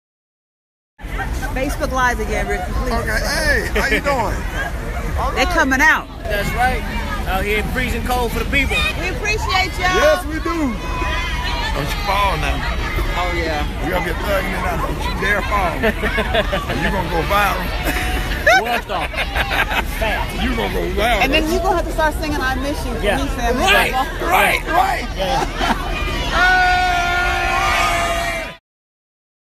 "How To Go Viral" MCC Parade w/Joyce Irby, Rickey Smiley & Sammie
:30 clip of fun with me, Rickey Smiley & Sammie on a float at the McDonalds Magic City Classic Parade in Birmingham, AL